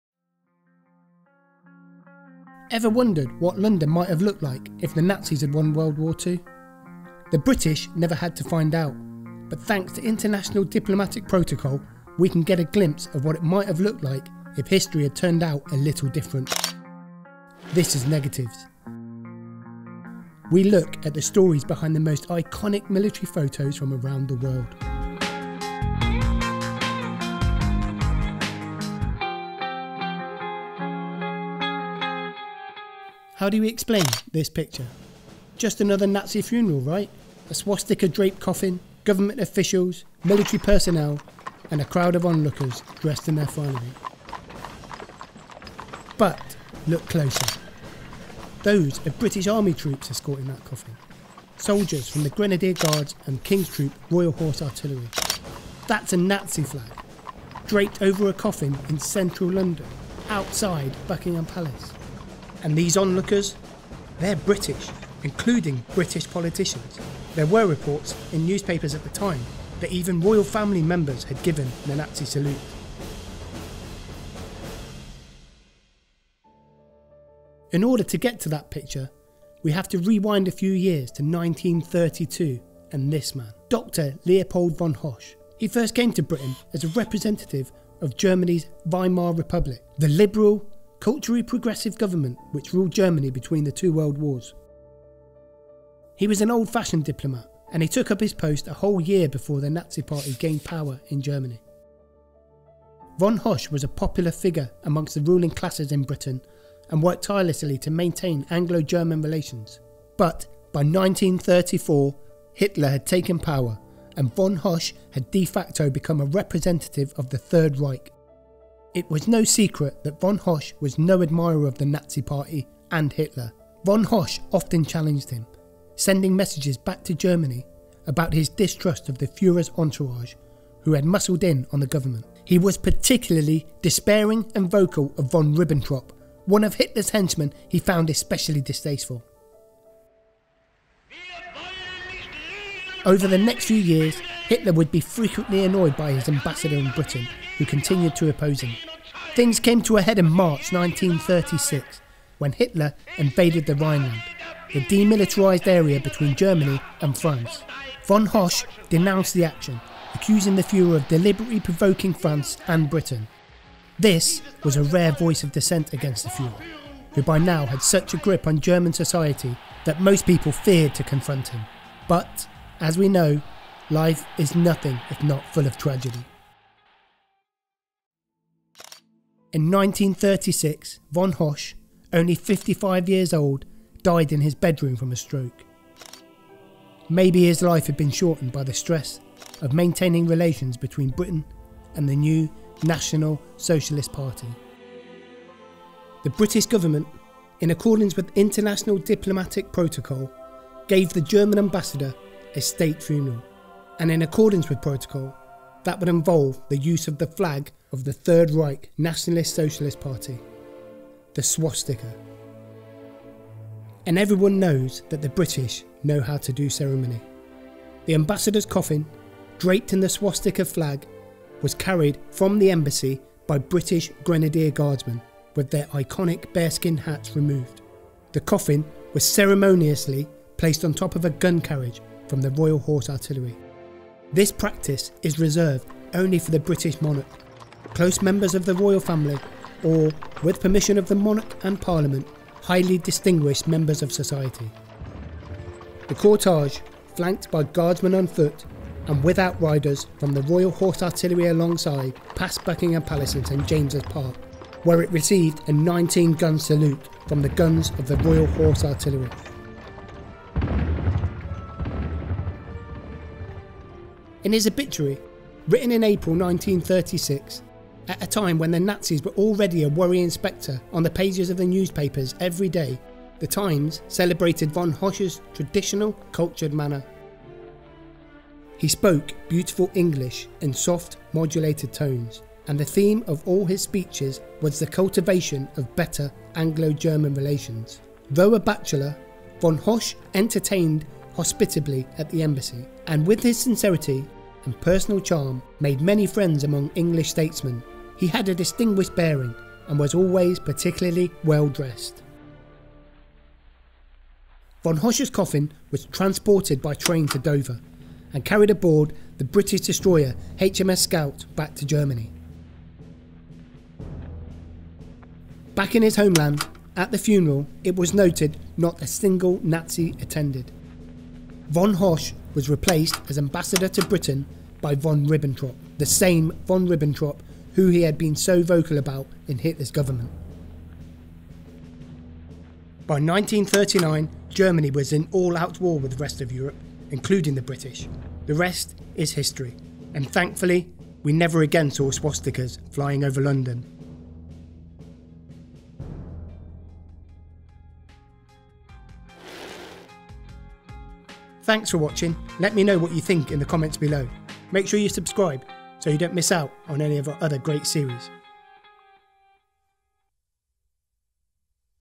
Why There Was a NAZI Funeral in London | NEGATIVES
The ominous Swastika flag of the Nazi Third Reich paraded through the streets of London, with full military honours and an adoring crowd.  Sounds like a horror movie.  But in 1936, when then German Ambassador to Britain, Dr Leopold von Hoesch, died suddenly at his residence in London, his funeral resulted in just such an event. The Swastika flag and Nazi Regime would go on to have a completely different meaning in history, and von Hoesch, who was openly vocal against the Nazi regime, would have hated to have been associated with it. This was a Nazi funeral, not the funeral of a Nazi.

Read more about the Ambassador and find out about the memorial to his dog, Giro, in London